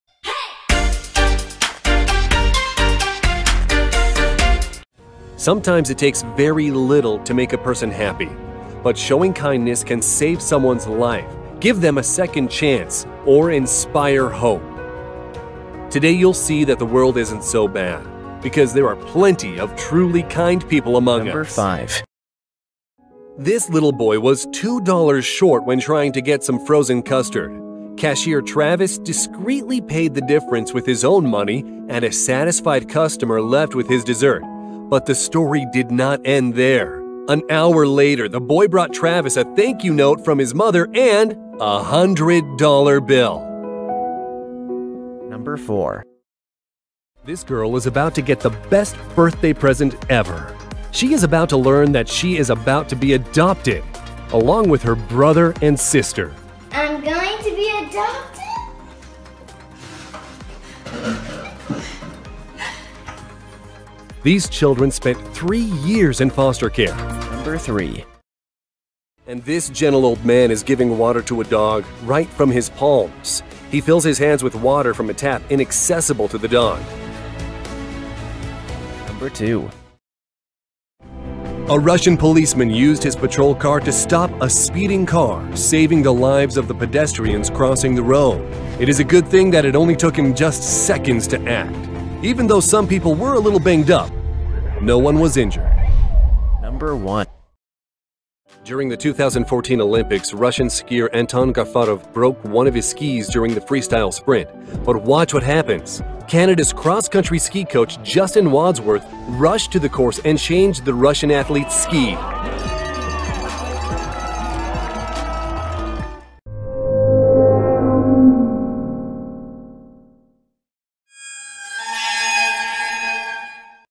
Top 5 Acts of Kindness
There are moments when we feel just like giving up on the world, on all the bad things that happen daily, but every once in a while we witness a random act of kindness that makes us believe that maybe there’s still a hope in this world, that there’s still people who really make a difference.